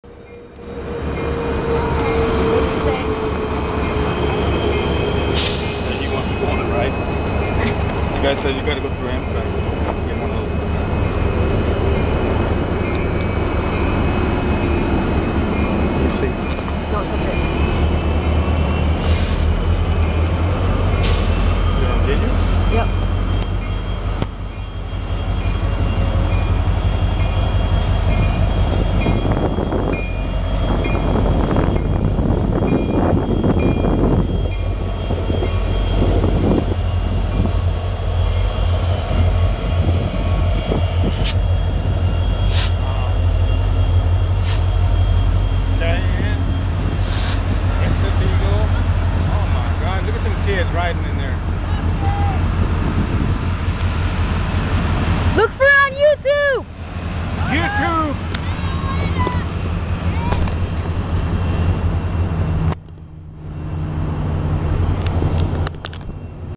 Union Pacific Special In tucson very very rare
UP SD70M #5054,UP SD70M #5052 and UP SD70M #5023 leading a UP Special through Amtrak Really really great catch!